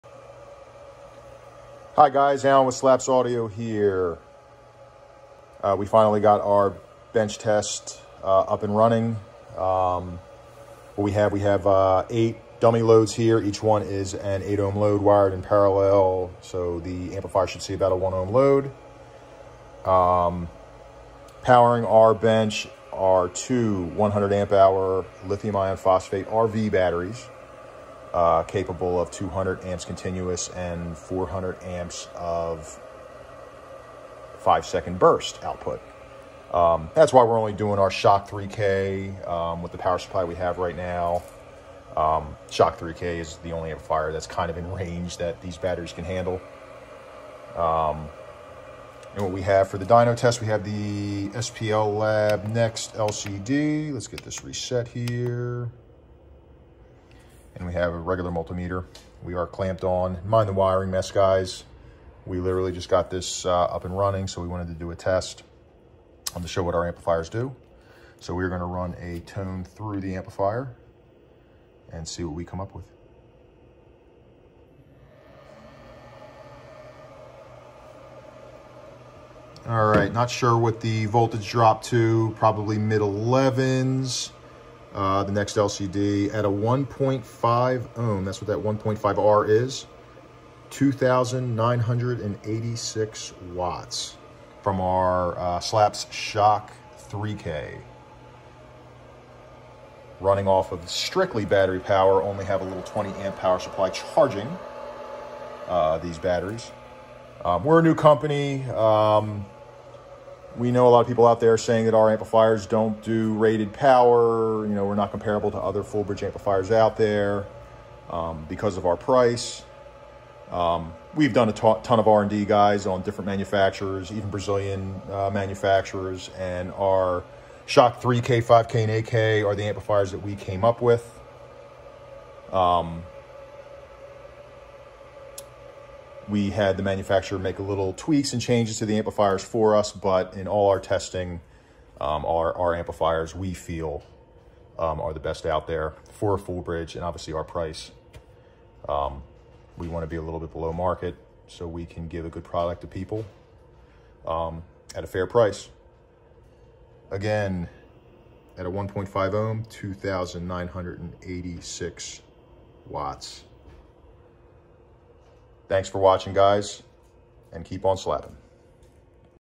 SLAPZ audio Shock 3k dyno test on spl lab next lcd
SLAPZ Car Audio shock 3k full bridge amplifier bench test on battery power only metered at 2986 watts at 1.5 ohm with voltage dropping to 11.7 volts on the SPL lab next LCD